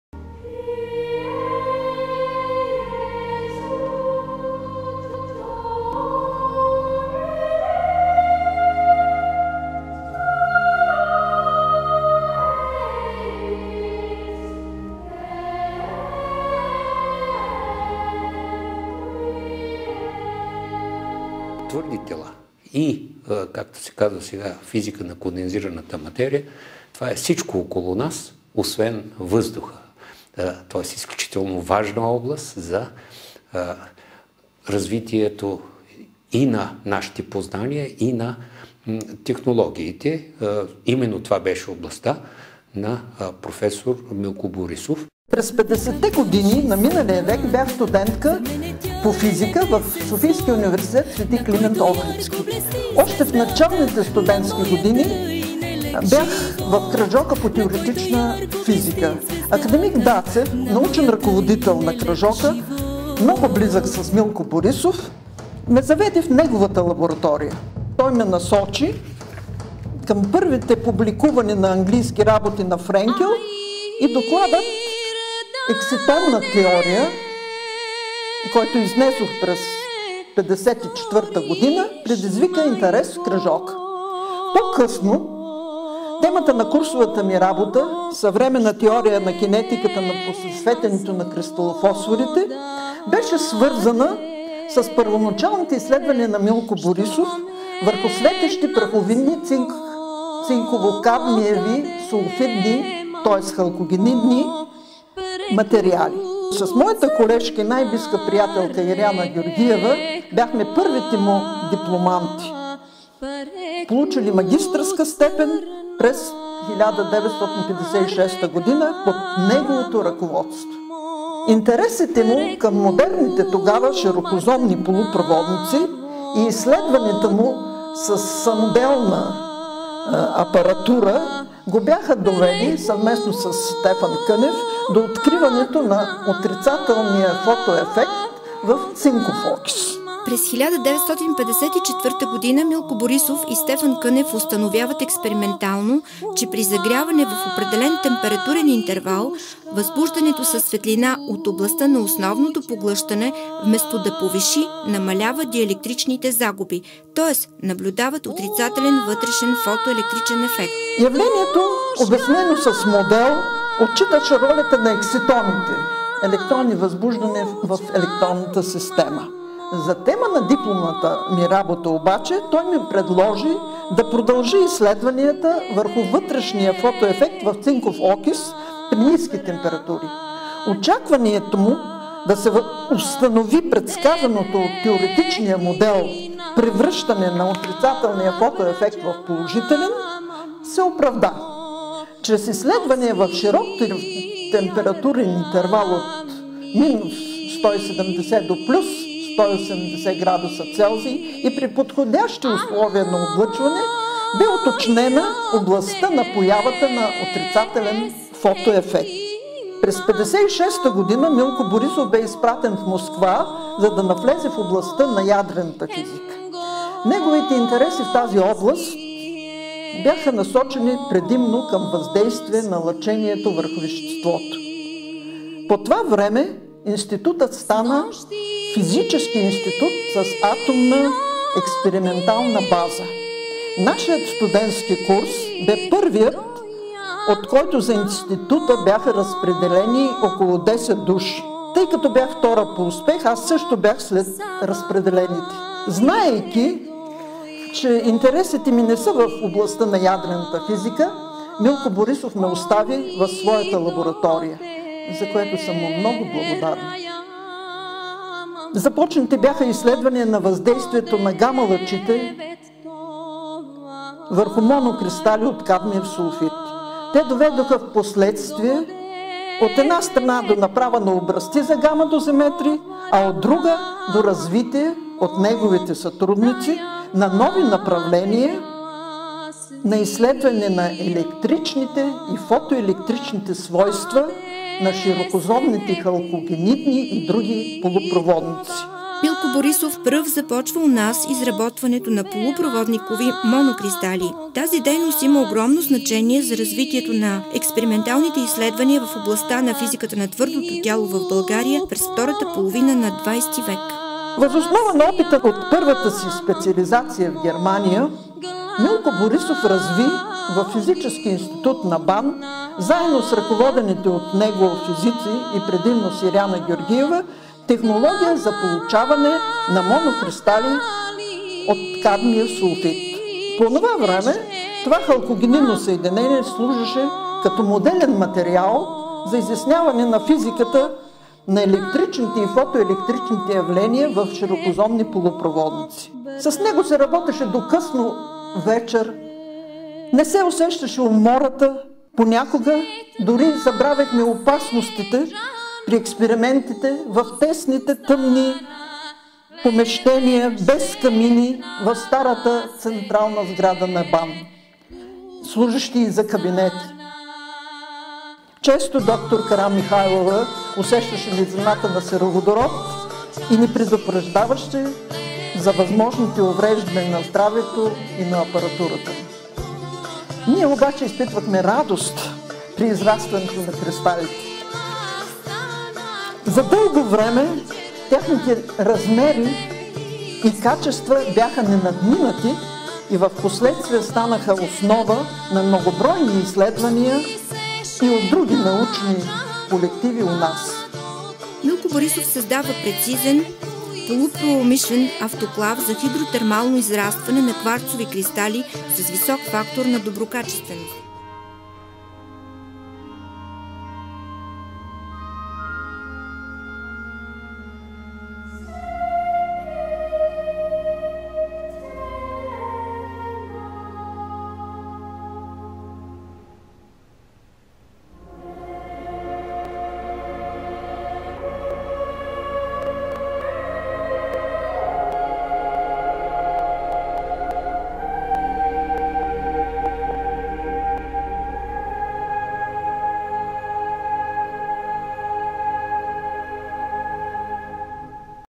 СПОМЕНЪТ ІV
Проф. дфн Иван Лалов и проф. дфн Елена Ватева разказват за работата си под ръководството на проф. Милко Борисов в Софийския университет. Филмът е създаден в Музея по история на физическите науки в България при Института по физика на твърдото тяло - БАН през 2009 г.